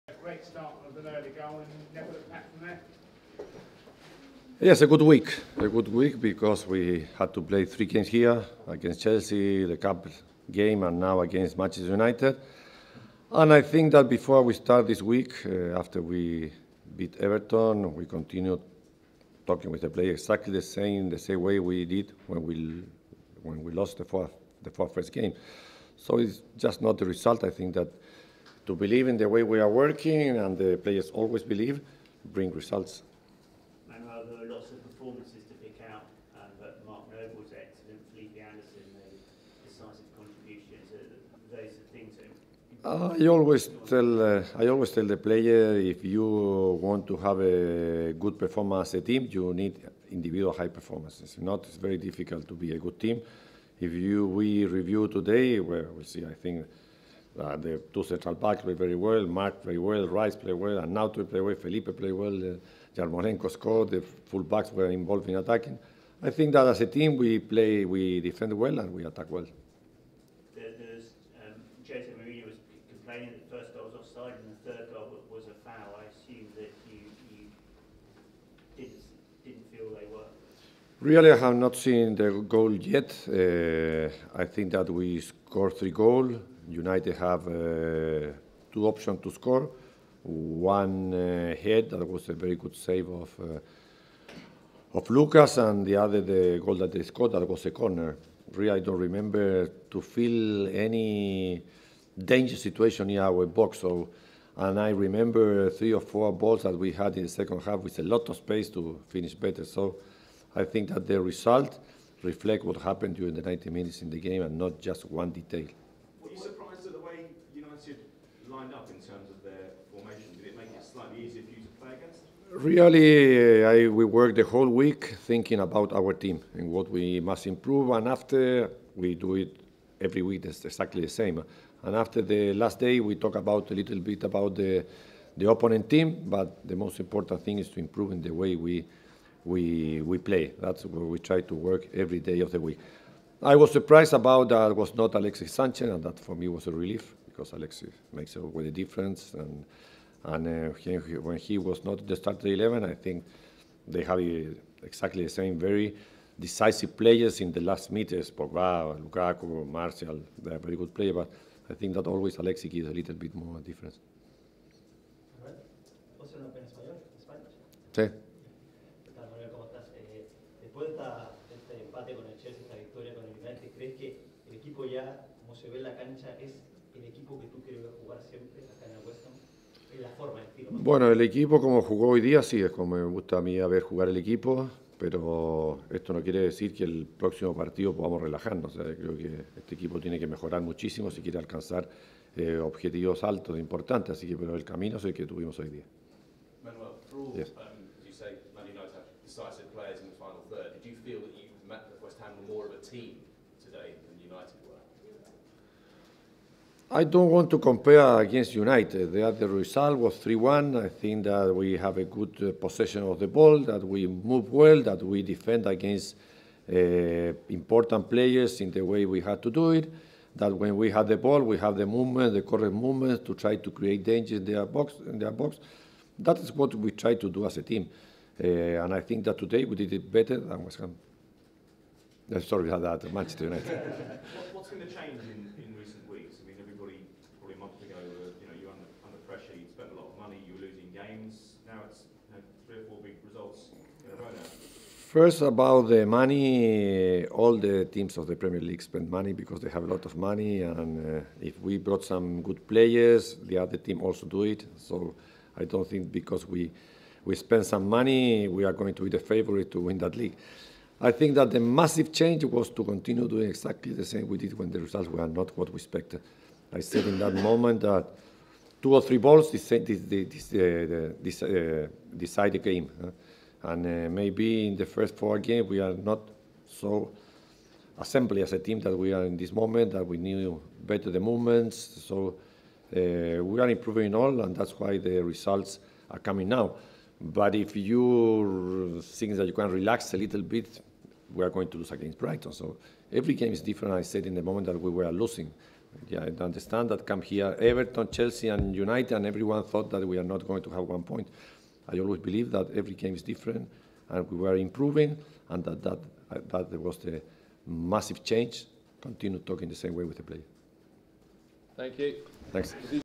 Manuel Pellegrini 'surprised' by Alexis Sanchez omission - West Ham 3-1 Manchester United
West Ham boss Manuel Pellegrini admits he was "surprised" to see Alexis Sanchez left out of the Manchester United squad by Jose Mourinho, after the Hammers heaped more pressure on the Reds with victory at the London Stadium.